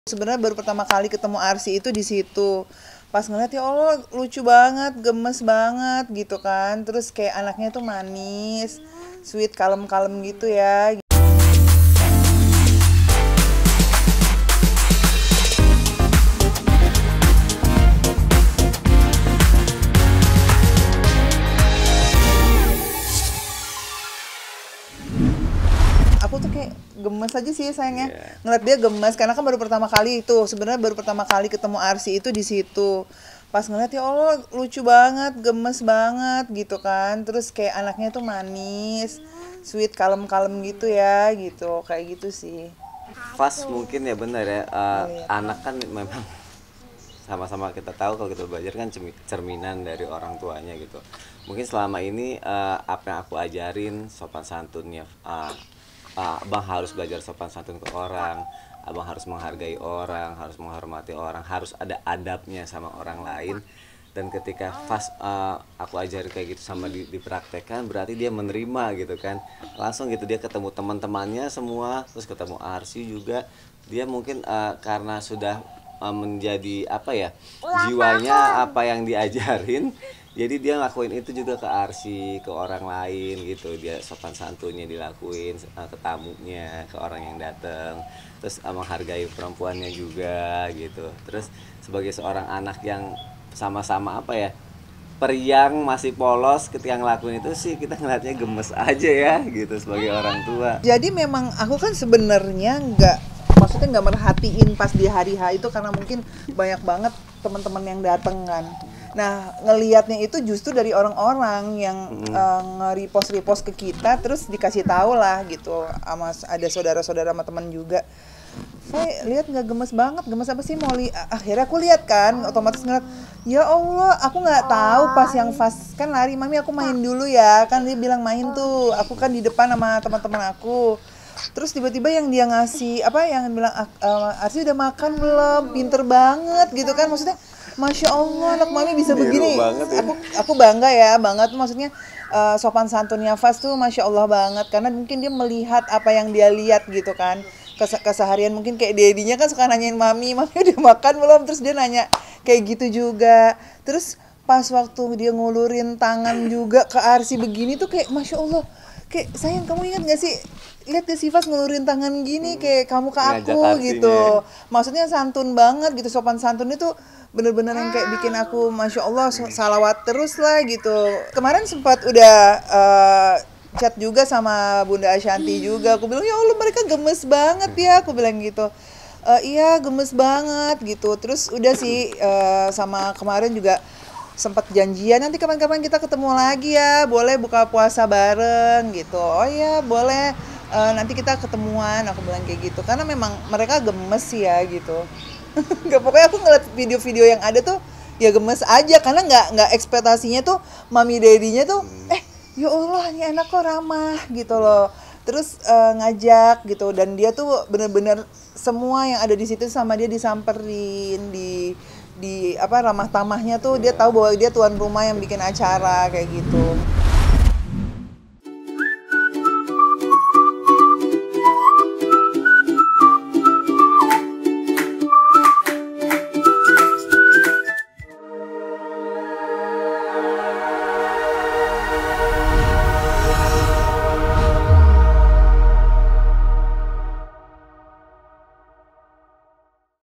Momen Manis King Faaz dan Arsy Bikin Fairuz dan Sonny Gemas
Momen ini terjadi saat putri Ashanty itu menghadiri pesta ulang tahun Faaz yang ke-10. Dalam kesempatan ini, putra sulung Fairuz A. Rafiq memperlakukan Arsy dengan sangat baik. Hal ini tentu saja membuat Fairuz dan Sonny Septian gemas sekaligus bangga dengan perlakuan putrannya.